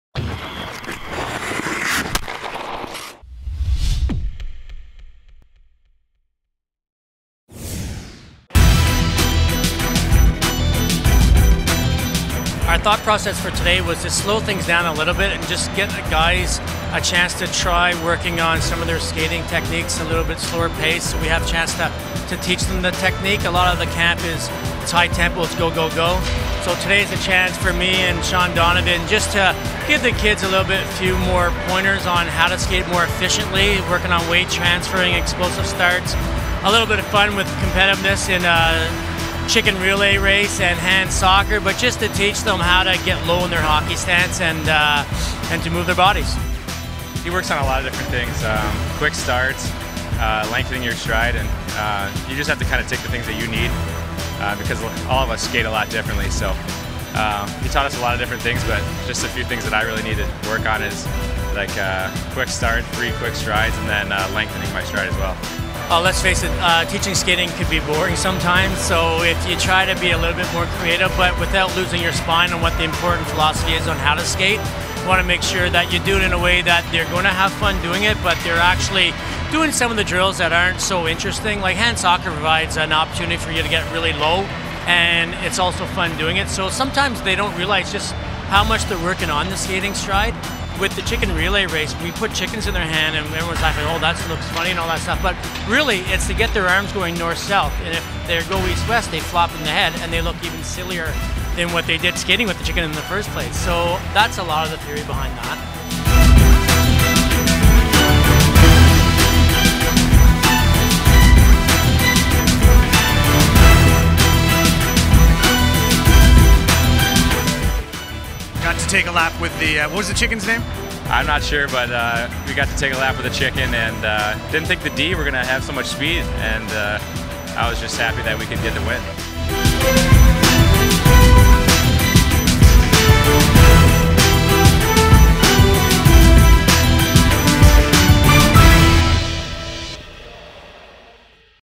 2015 Dev Camp Skating Techniques
Sens Dev Camp takes the prospects to the Sensplex with skating coach Marc Power for various skating drills and exercises.